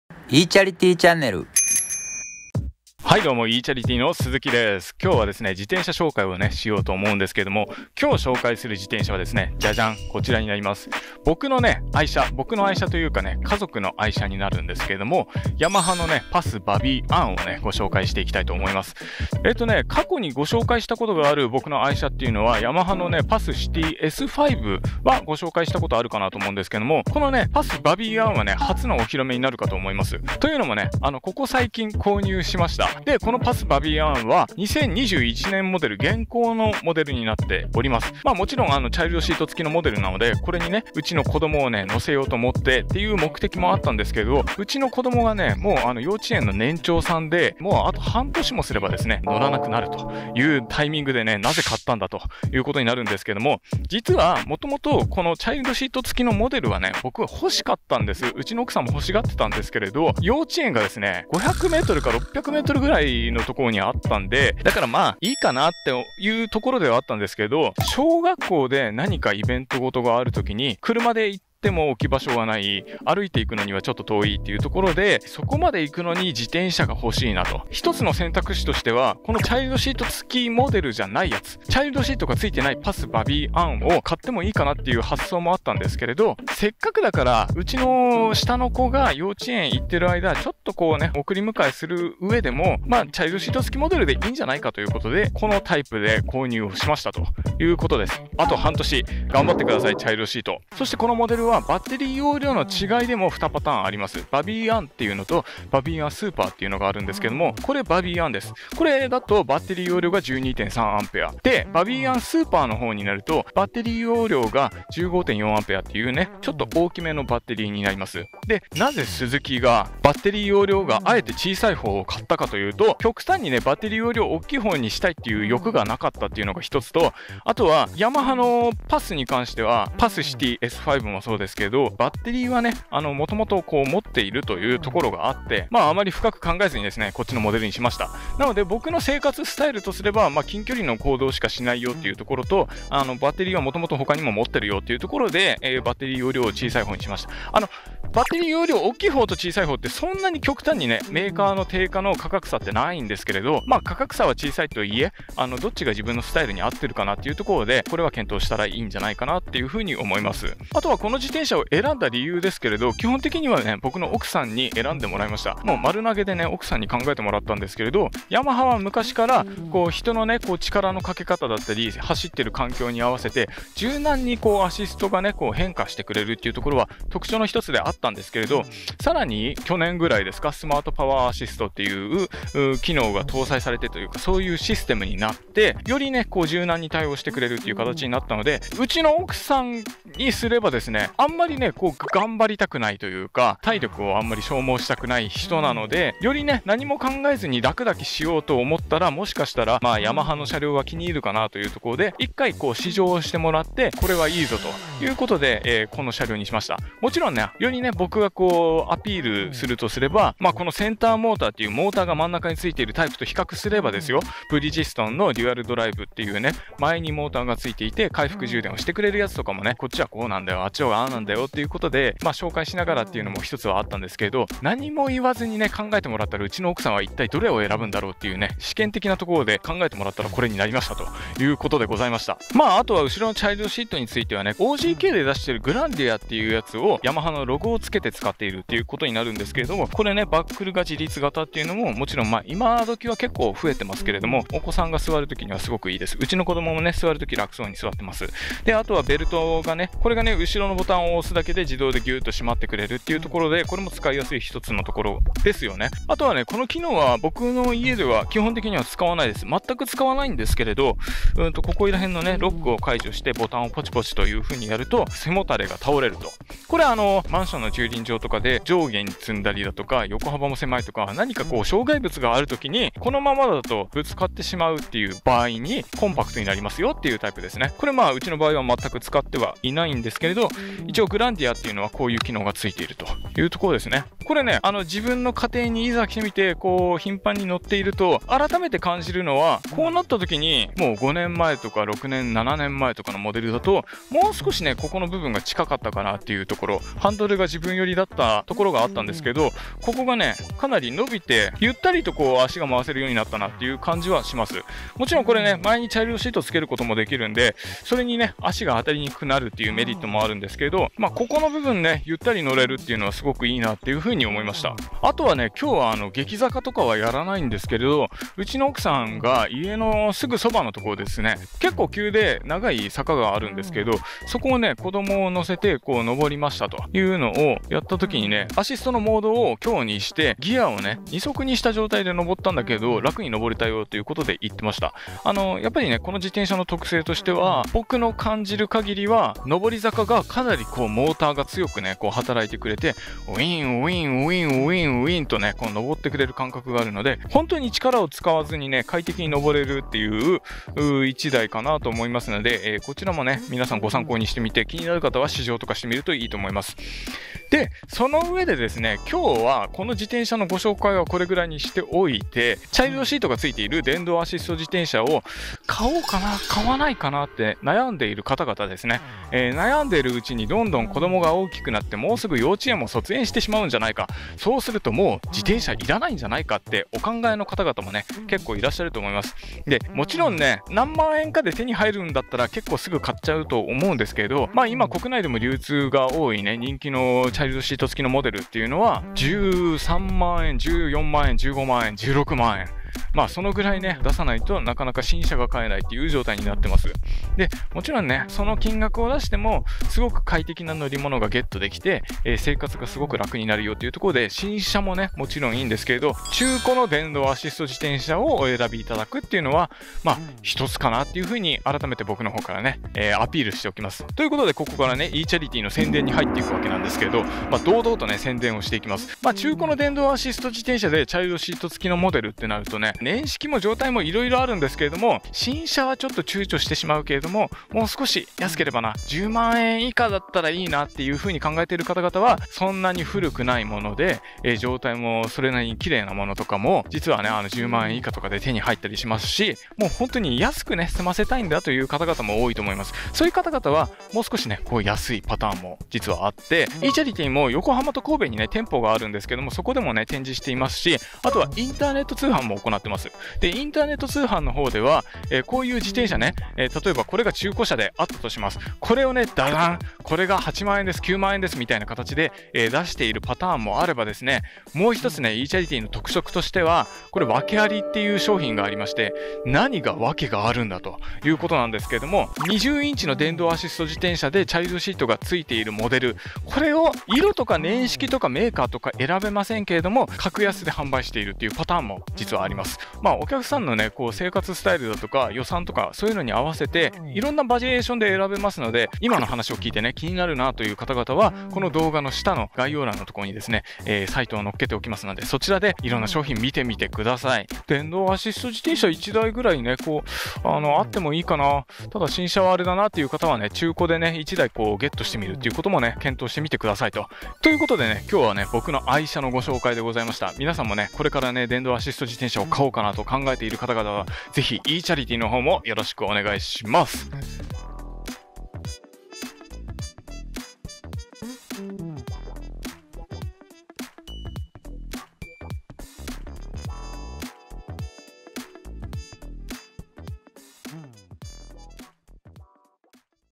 【電動アシスト自転車】専門家の妻が選んだのは、ヤマハのパスバビーアン。（電動自転車/子供乗せ/子乗せ/YAMAHA/PAS/BABBY UN/中古/インプレッション/比較/紹介/試乗/2021）
オフィシャル通販サイト

電動自転車サブスクリプションサービス始めました【最安月額¥980～】

ご不要な電動アシスト自転車の買取はこちら